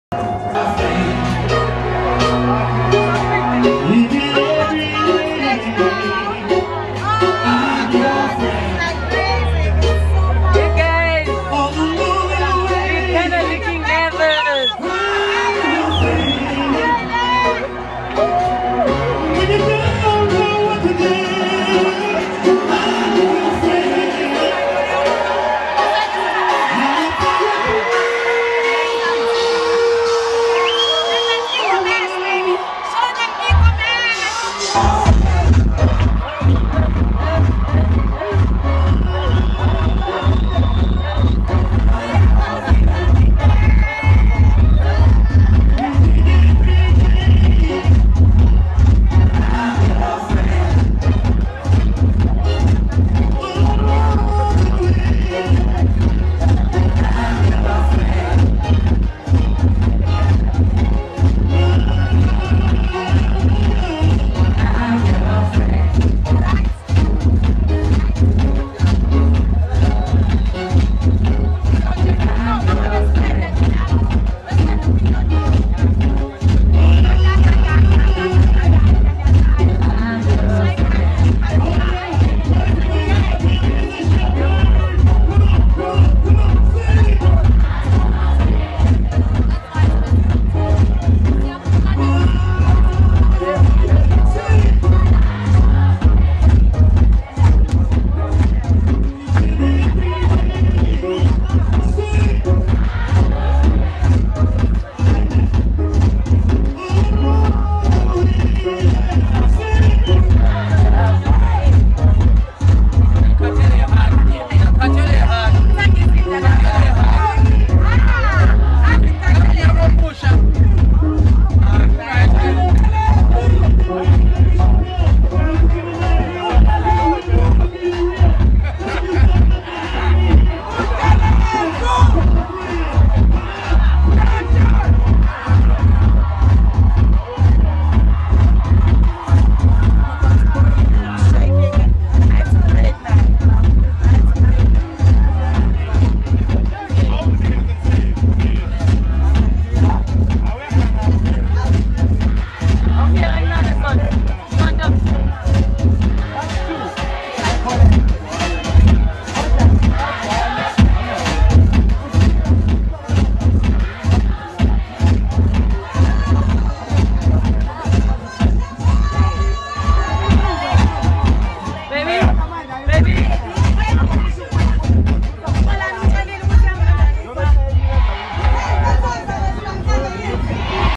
Backstage with Queen Londiwe
Backstage footage of Queen Londiwe with her dancers and fans at Richard's Bay Festival.